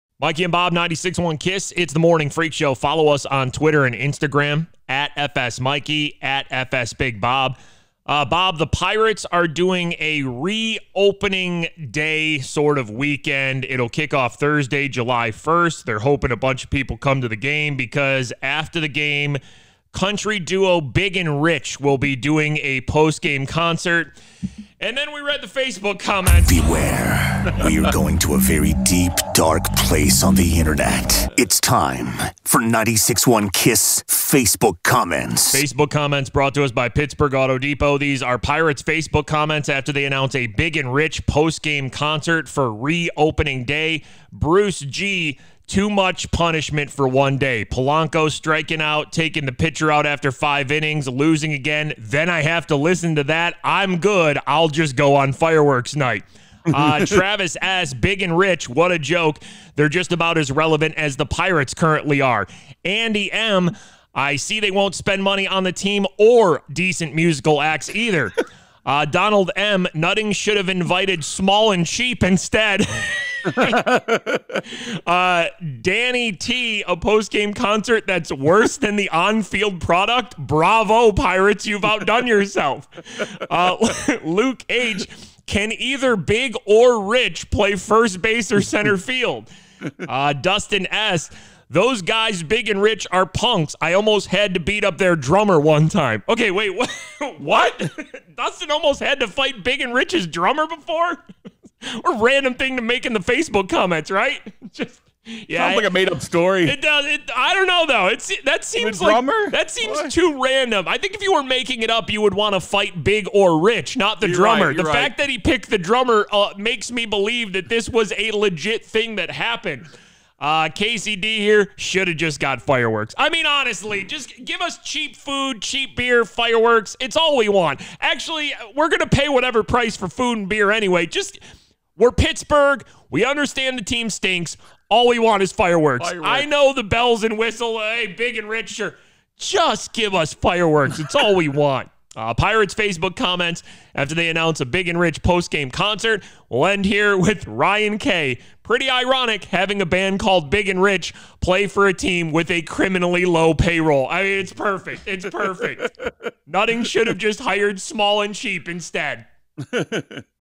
Big and Rich postgame Pirates concert Facebook comments
We read the Pirates Facebook comments about Big and Rich playing a postgame concert